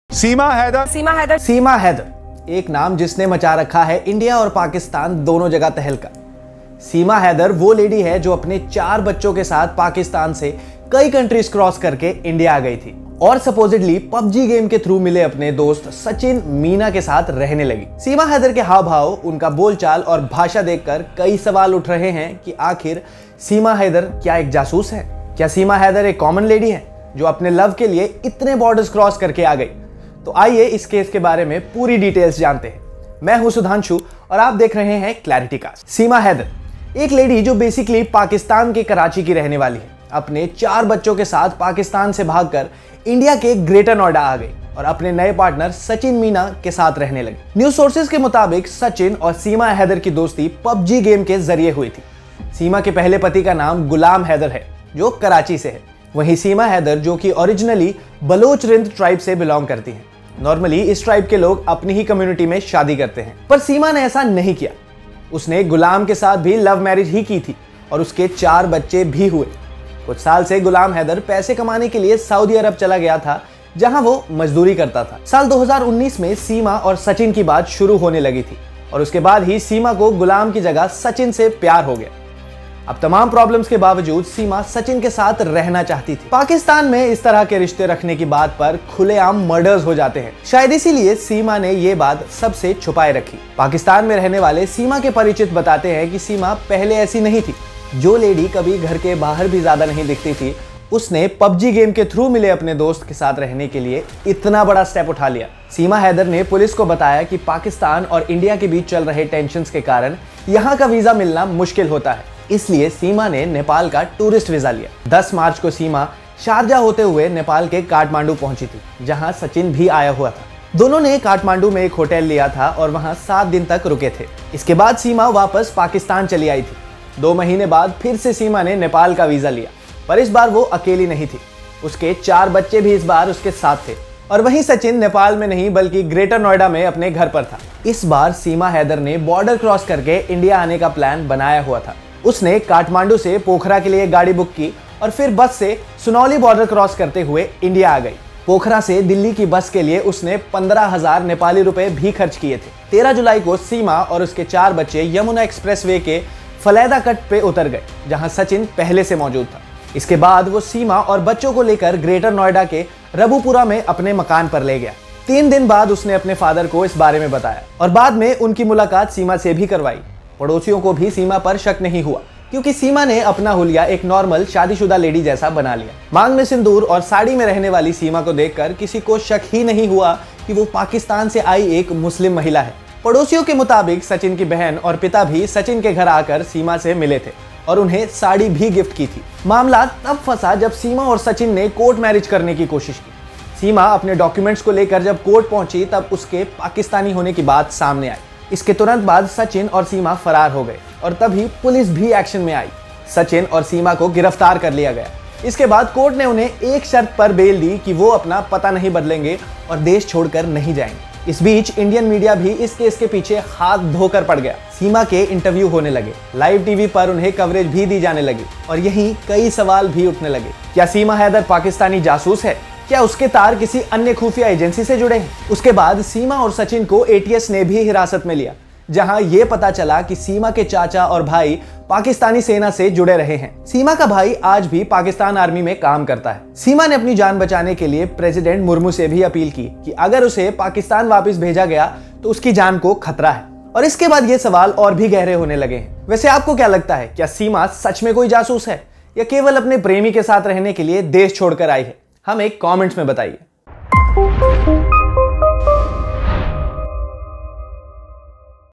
Is Seema Haider really a spy? | ClarityCast
Seema Haider - The lady who has crossed borders for his lover Sachin Meena and has brought all her children from Pakistan is the latest sensation for the Indian and Pakistani Media. The extreme buzz around Seema is constantly posing the same question: Is Seema a spy?
Let's check out the full story of Seema Gulam Haider from Pakistan.

Out Of Syllabus is now Clarity Cast.